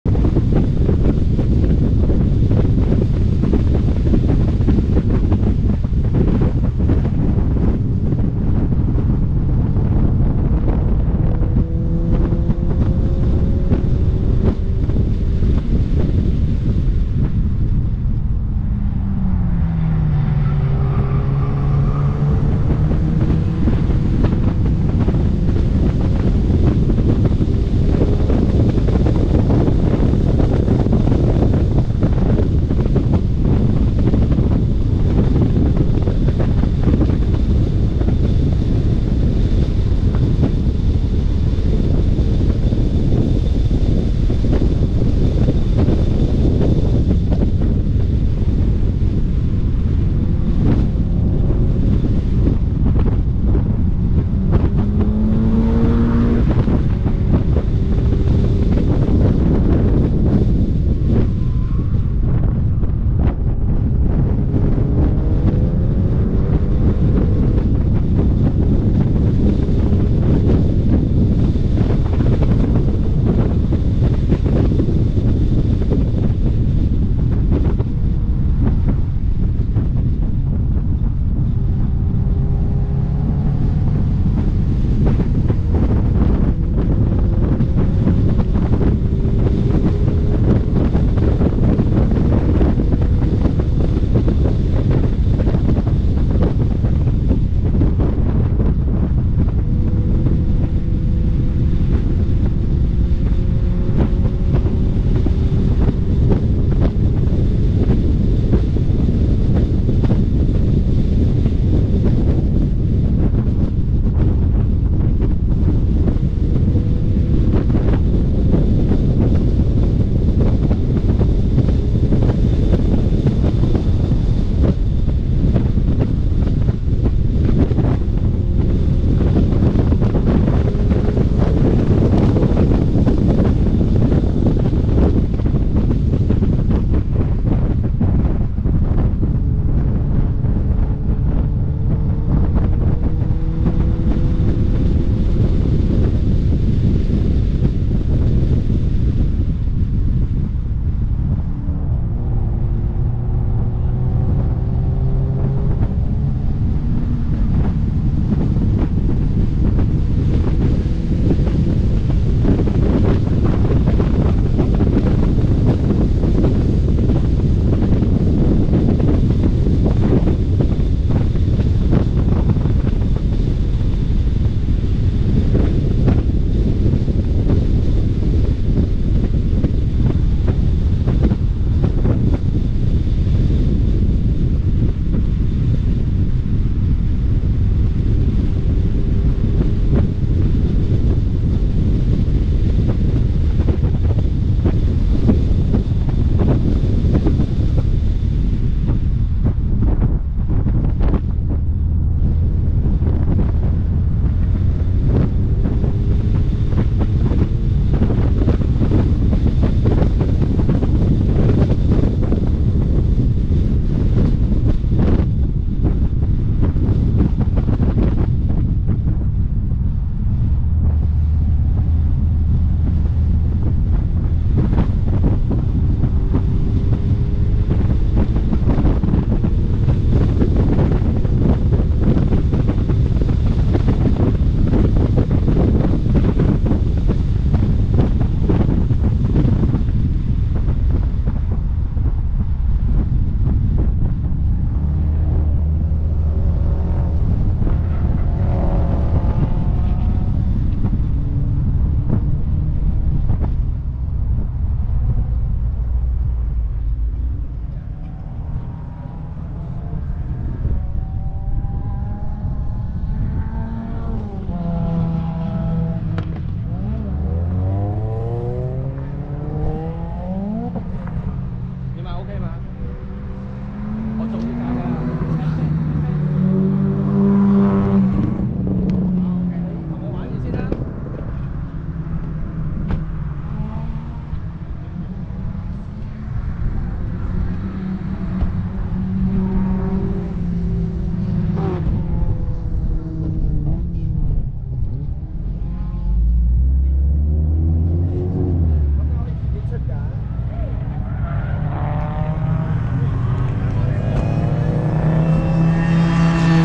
Red KIA Stinger - 360 footage @ 2022.8.1 Touge.ca Mosport DDT track event
Red KIA Stinger - 360 footage @ 2022.8.1 Touge.ca Mosport DDT track event  GS020199